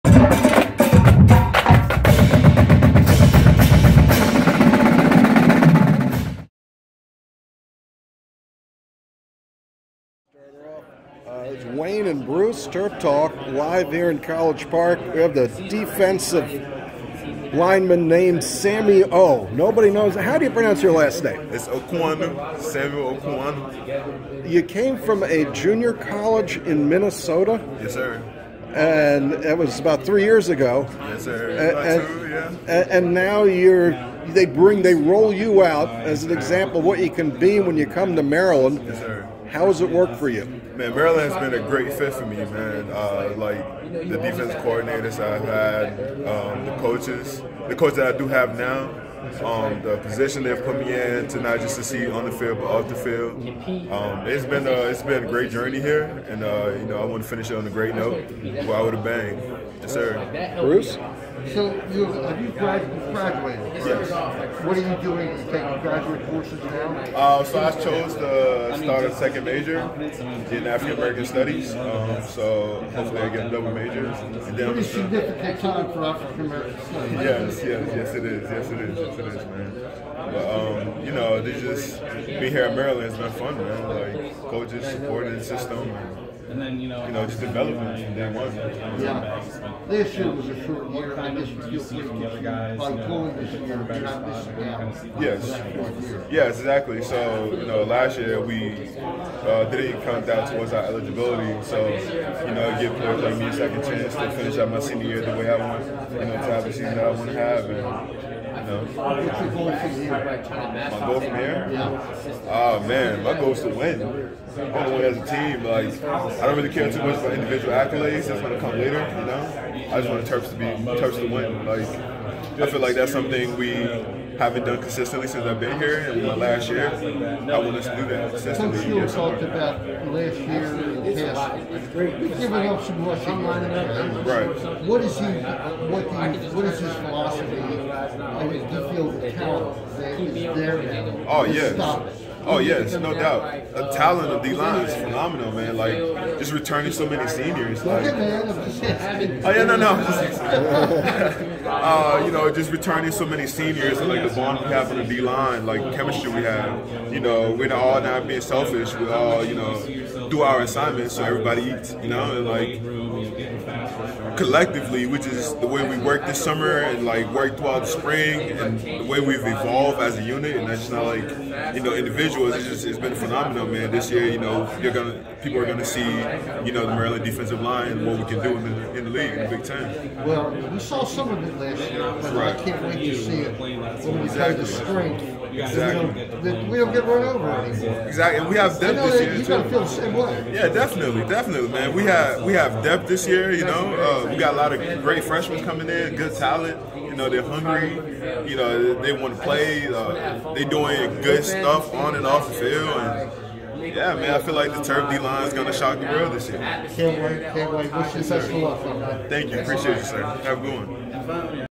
Maryland football 2021 - Sammy Okuayinonu Defensive Line
Sammy O on his Maryland journey from a juco in Minnesota to College Park and a double major.  With Wayne Viener + Bruce Posner